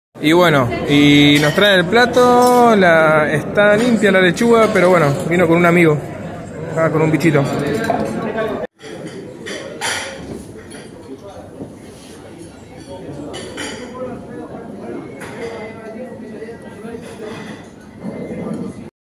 ROSARIO: Son más de cien los federales intoxicados hasta el momento
Casi una treintena debieron ser trasladados al Churruca y están fuera de peligro en tanto otros menos complicados también sufren síntomas. Ya había quejas sobre la comida que recibían pero no hubo reacciones. LA PROVINCIA DE SANTA FE NADA HA DICHO A PESAR DE QUE ES LA CONTRATANTE DEL CATERING.